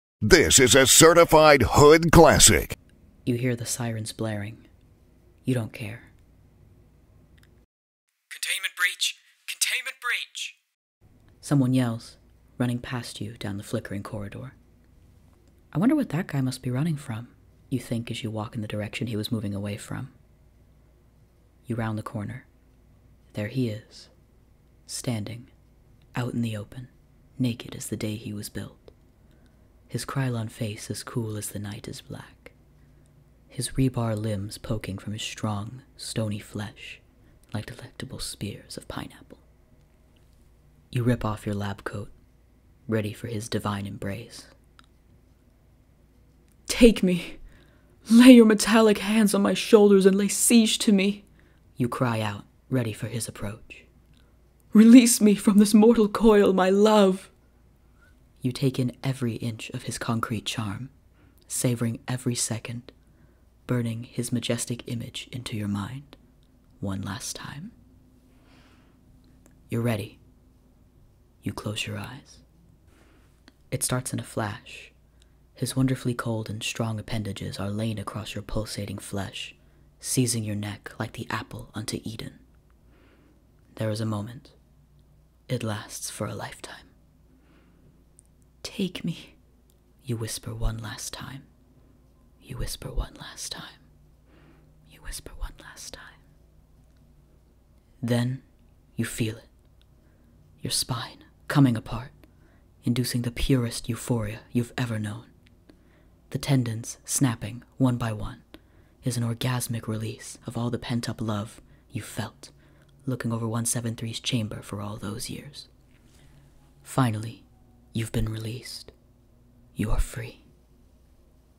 [SCP Wiki Reading] Taken by the Statue
I was futzing about on the SCP Wiki, and found a Reader x 173 fanfiction published to the official site. I simply had to dramatic read it.